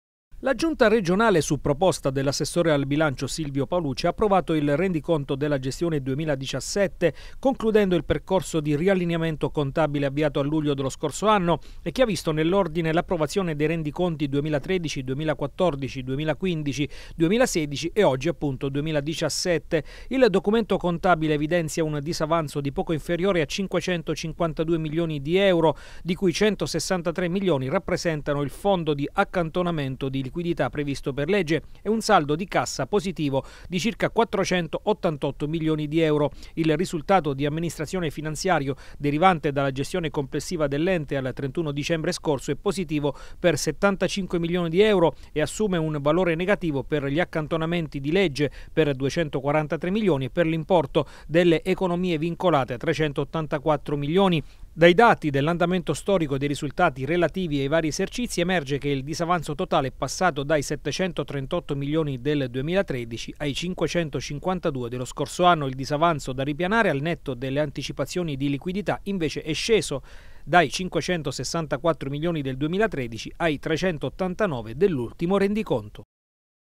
Bilancio Regione approvato il rendiconto 2017
Approvato in giunta il rendiconto finanziario della Regione per l’annualità 2017, che conclude il percorso di riallineamento dei conti dell’ente